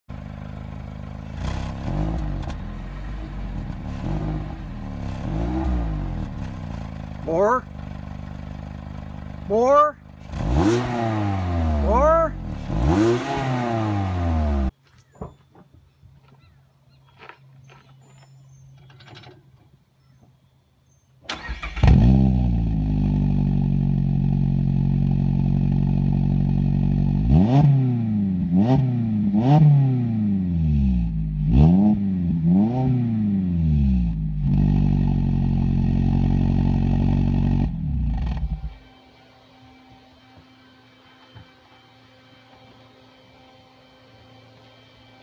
Super Street Muffler vs. Stock 2017 RF Miata
Before and after install of Goodwin Super Street muffler on 17 RF